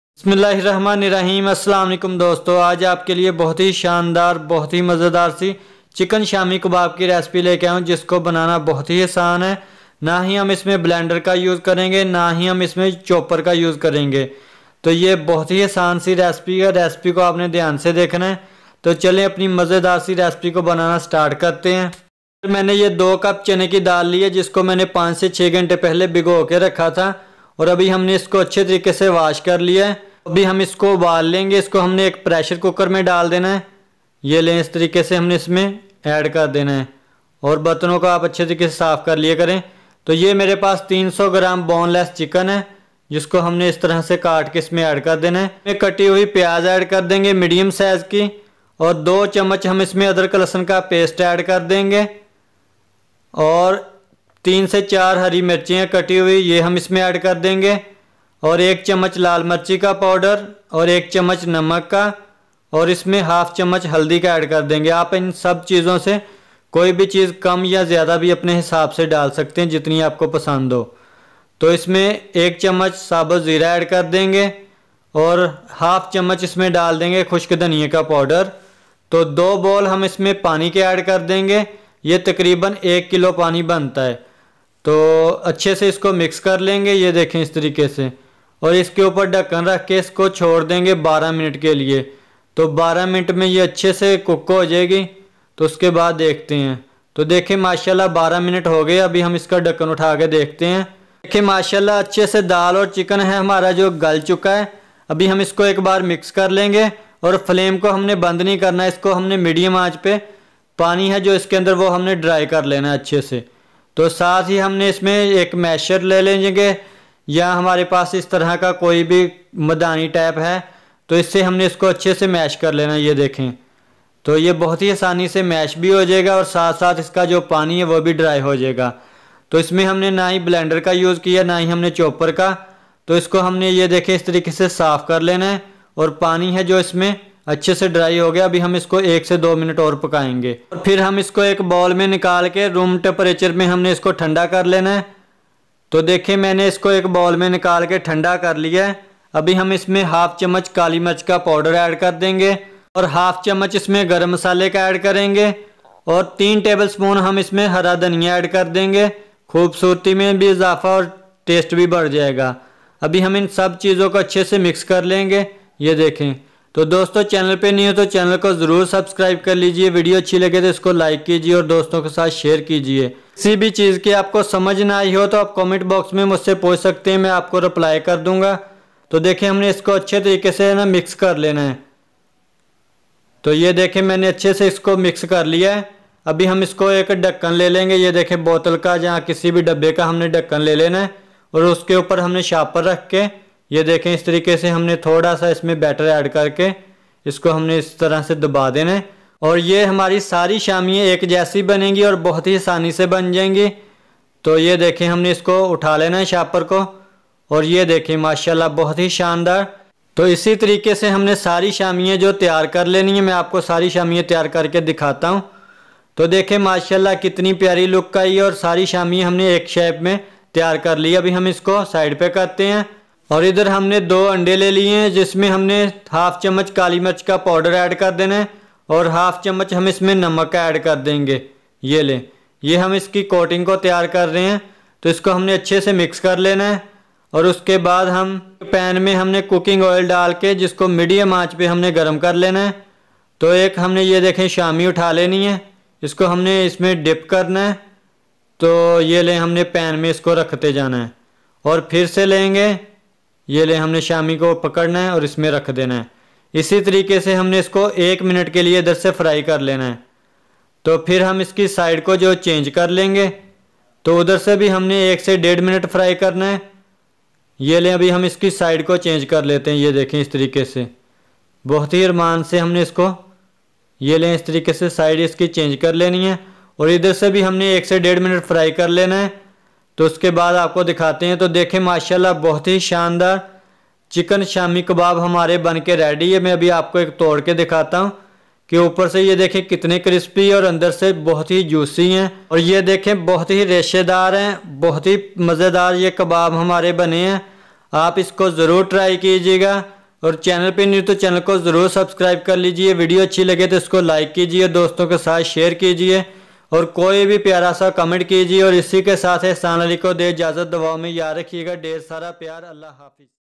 Chicken Shami Kabab |چکن شامی کباب بنانے کا طریقہ | Dal Chicken Tikki | kabab banane ka tarika
aj me ap k ly lya hun buhat hi mazedar si Chicken Shami Kabab ki recipe | چکن شامی کباب بنانے کا طریقہ | Dal Chicken Tikki kabab banane ka tarika Ramadan Special Recipes